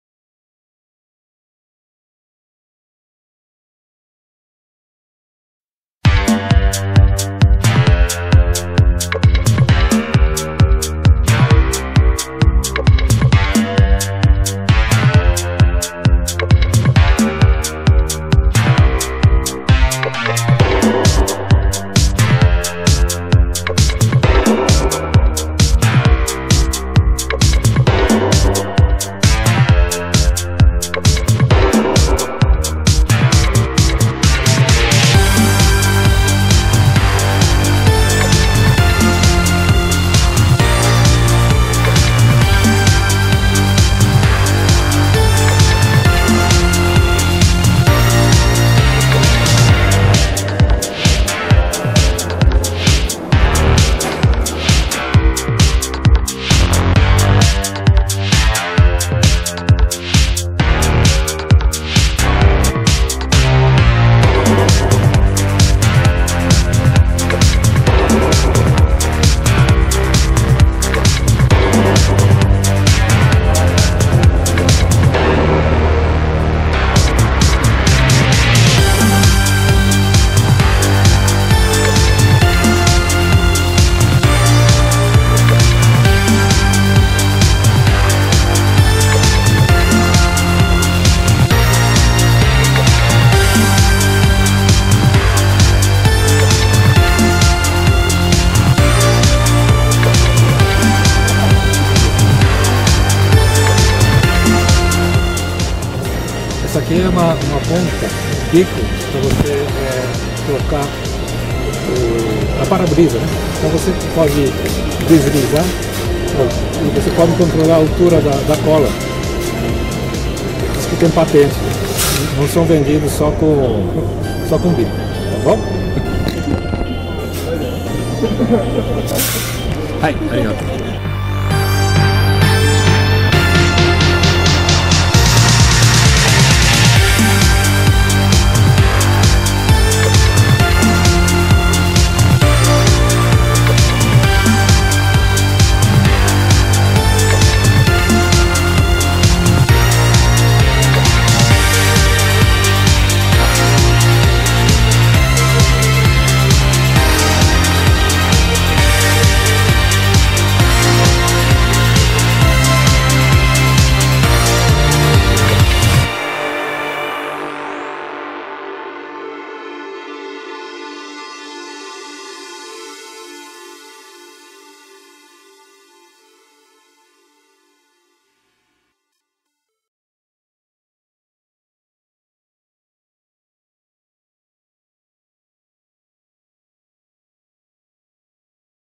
1098 Sprex Japan - Auto Service Show 2019 - 5
O último vídeo de Auto Service Show mostra estandes de empresas de Detalhamento, Reparo de Vidros, Jateamento de Gelo Seco, Ferramentas Manuais, Massa de Calafetar e Colas. E antigo parceiro da Sprex Japan - Cursos e Ferramentas MS Japan Service que representa máquinas, equipamentos da Spanesi, Cebora S.p.A. e outras marcas no Japão.
Quem quiser aprender técnicas de Funilaria e Pintura no Japão, entre em contato com Gerson Massao pelo WhatsApp ou visite oficina abaixo.
Oficina: Car Convenience Club TAIHEI
3-72, Mangoku, Ogaki-shi, Gigu-ken, Japão